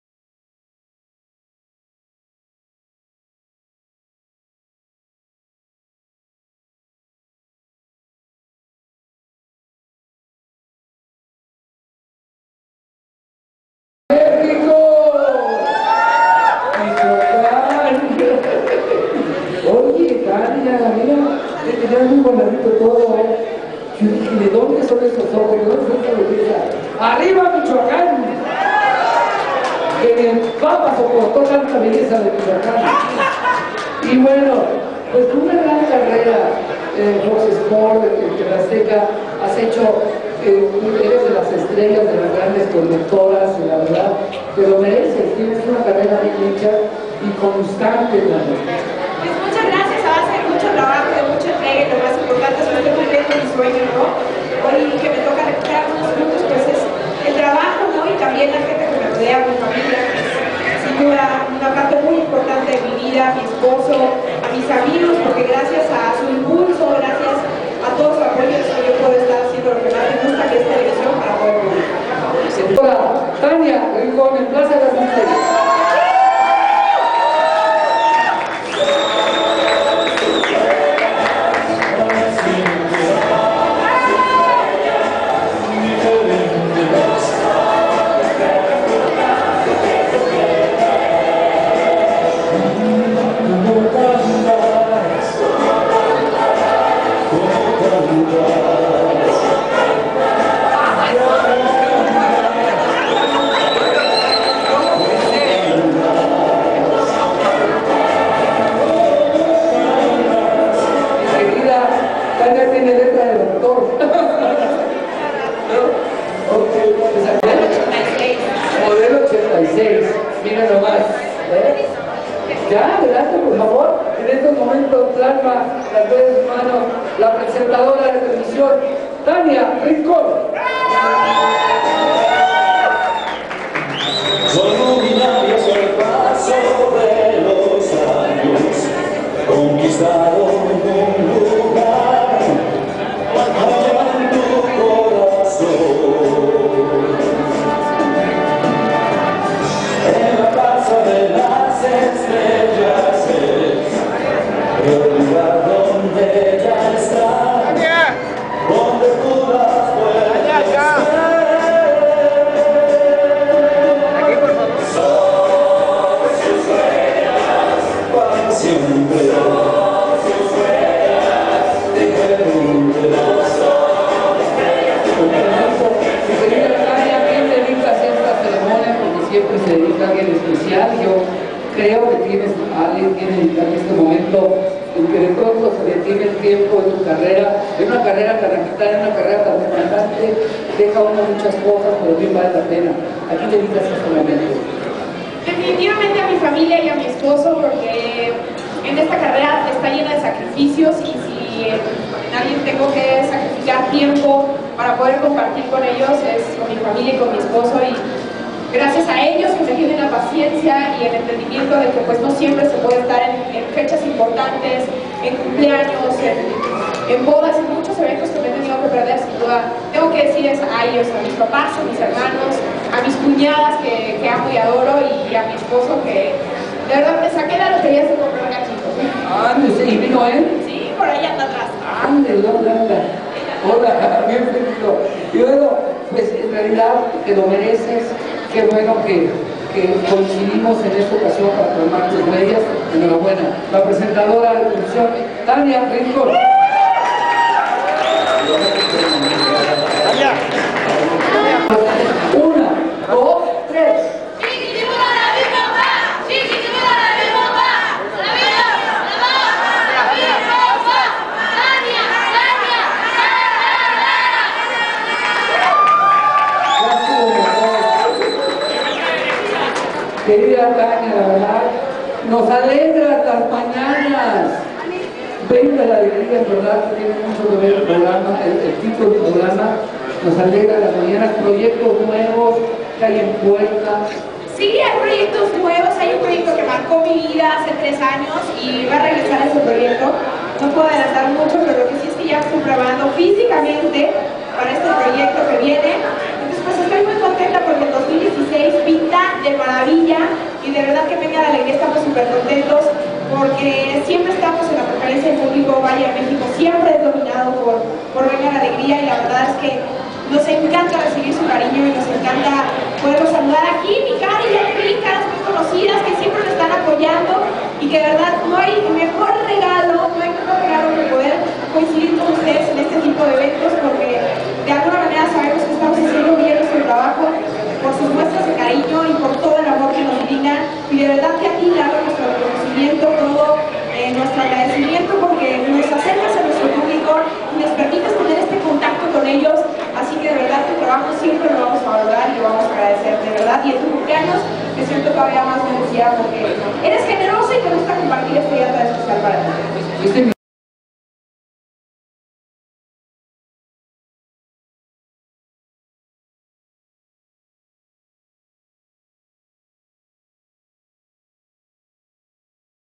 TANIA RINCON
Plasmó las Huellas de sus manos la Conductora de Televición y Ganadora del Certamen " Nuestra Belleza Mexico 2016 " TANIA RINCON  en Galerias Plaza de las Estrellas CDMX video tomado y editado por: Ruben Gaeta